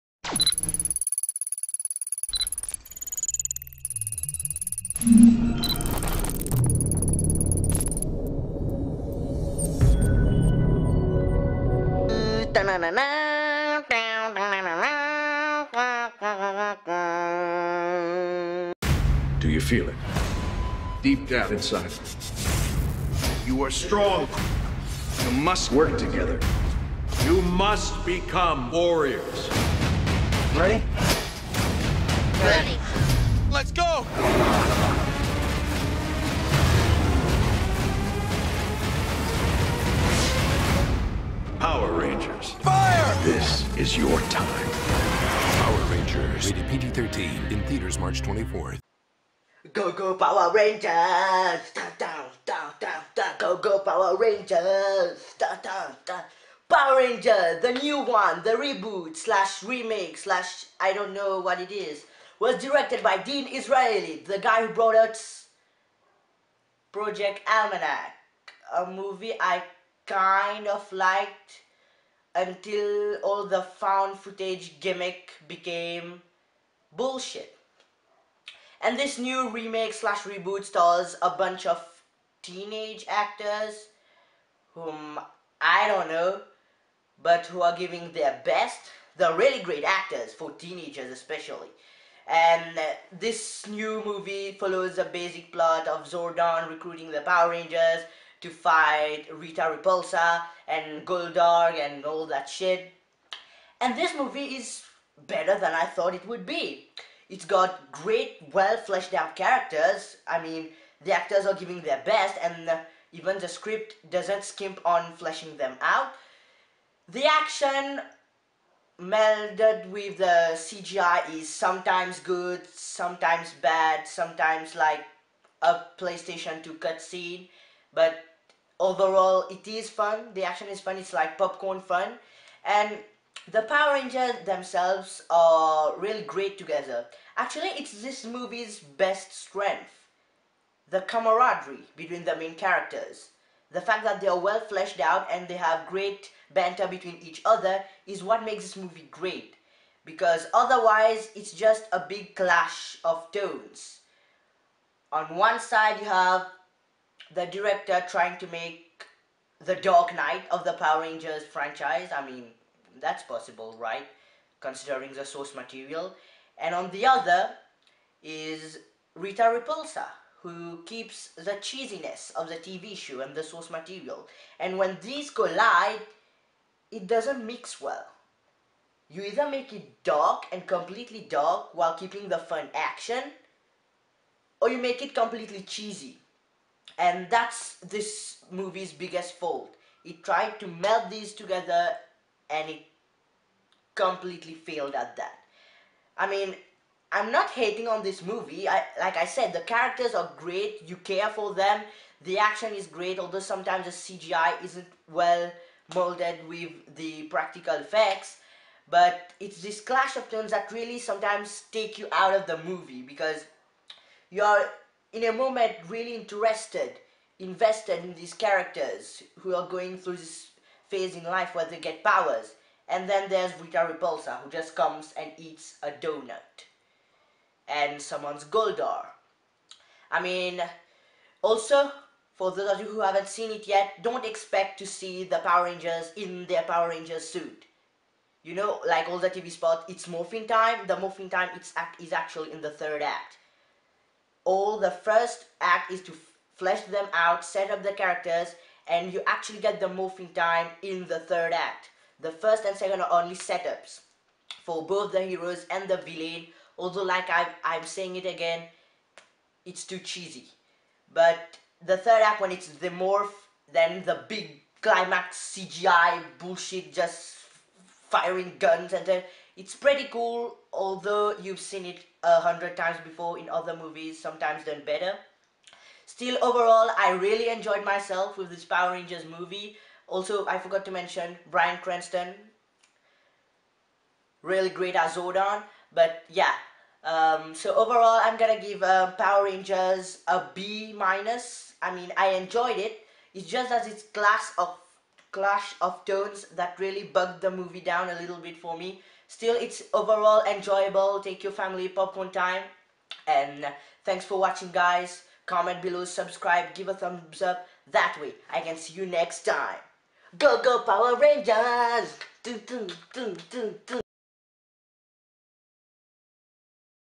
Power Rangers - Movie Review
Krishen Seethiah reviews Power Rangers, starring Dacre Montgomery, Naomi Scott, RJ Cyler, Becky G, Ludi Lin, Bill Hader, Bryan Cranston and Elizabeth Banks. Directed by Dean Israelite.